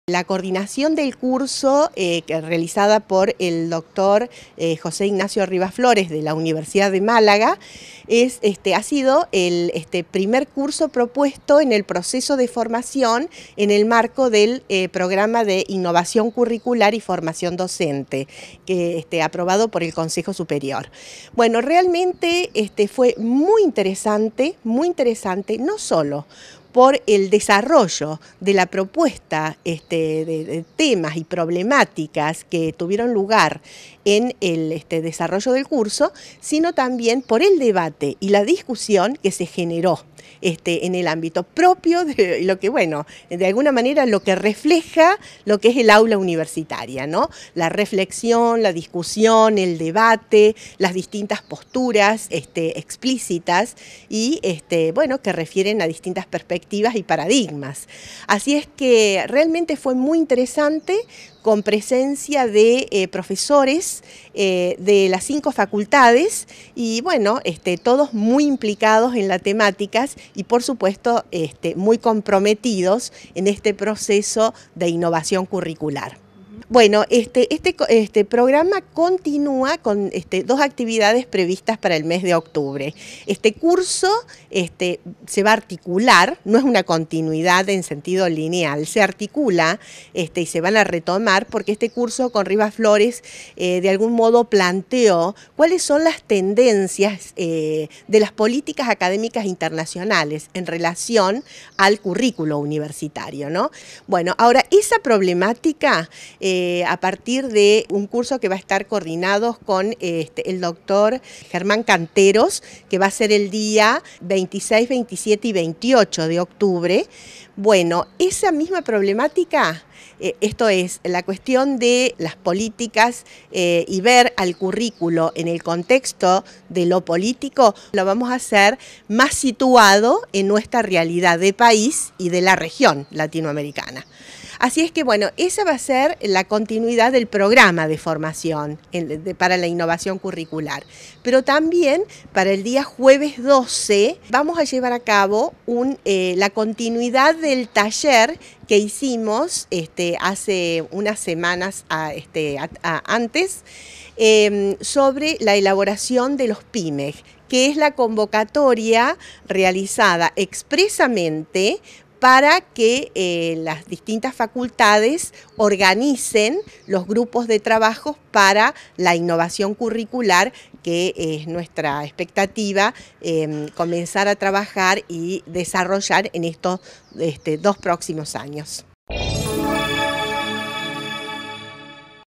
PRACTICAS PEDAGOGICAS Y FORMACION DOCENTE - ANA VOGLIOTTI
BALANCE POSITIVO DE LAS ACTIVIDADES COORDINADAS POR EL PROFESOR RIVAS FLORES
Facebook.com/uniriotv
Twitter.com/uniriotv